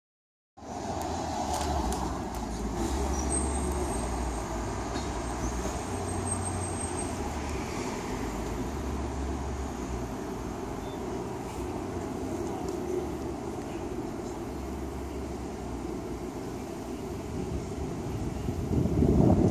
20190918 160612 Mercedes (W222) AMG S63 Renntech
Matte finish W222 Mercedes AMG S63 Supercar accelerating flyby. Blocked partly due to a crossover in the visual.